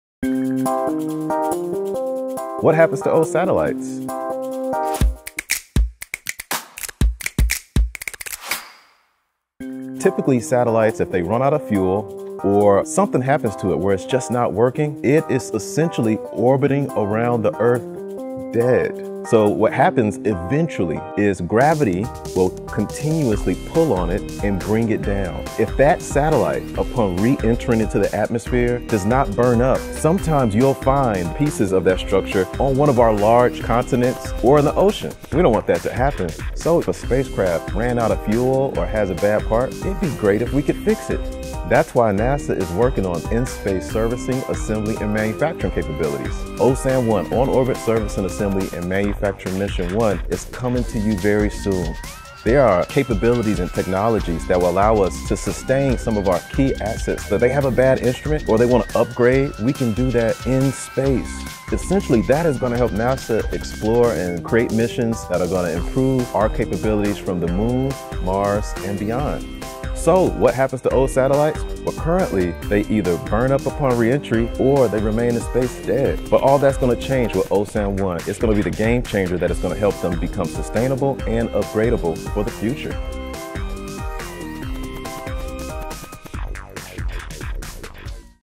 What Happens to Old Satellites We Asked a NASA Expert.
What happens to old satellites? Currently, they either burn up safely upon reentry into the atmosphere or they remain in space. But NASA is working on new technology that could make spaceflight more sustainable by refueling or upgrading satellites in space, greatly expanding their lifespans.

Here's more about the On-orbit Servicing, Assembly and Manufacturing 1 (OSAM-1) Mission